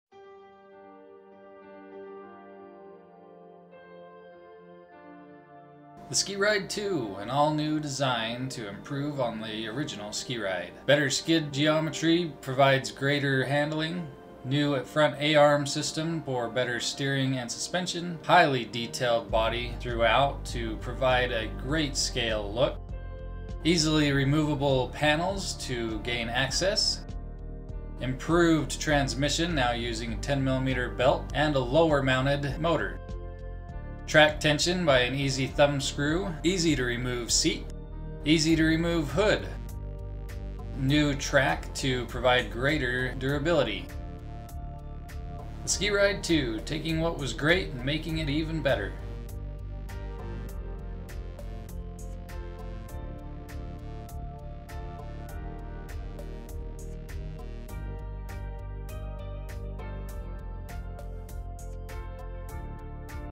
SkeeRide 2 New Features
Available Now!
Thank You All for the amazing support. Enjoy!

People that will print one for you found on this list

The SkeeRide 2 is a complete new design.
Taking from the knowledge gained from the
SkeeRide and all of the community feedback,
the SkeeRide 2 has been strengthened and
improved in all aspects of the design. You can
expect better durability, smoother suspension,
and greater performance. Due to these
changes the build will require more parts and
may be considered a more complex build in
comparison to the original SkeeRide.